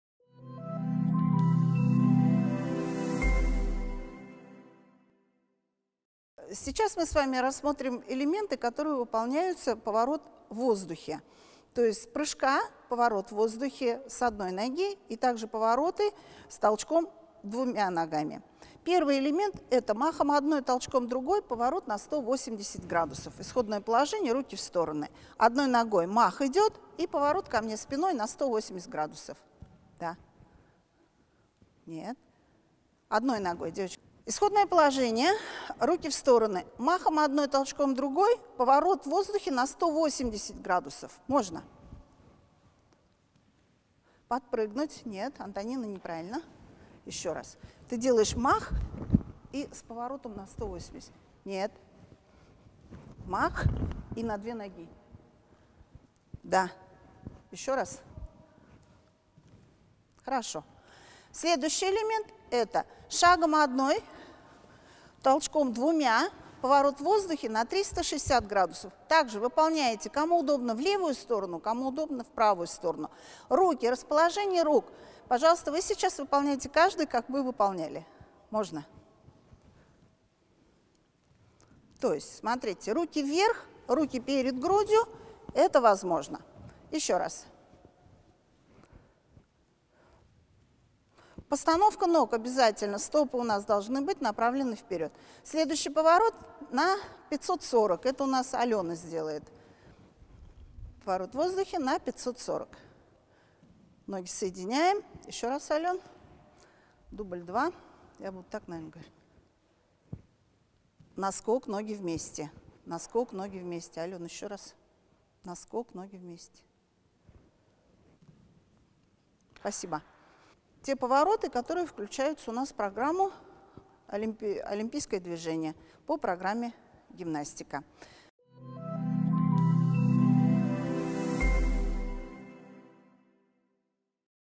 Урок по гимнастике-3
Уроки по гимнастике проводит Римма Мухаметовна Имамова, доцент кафедры теории и методики физического воспитания и спорта БГПУ им.М.Акмуллы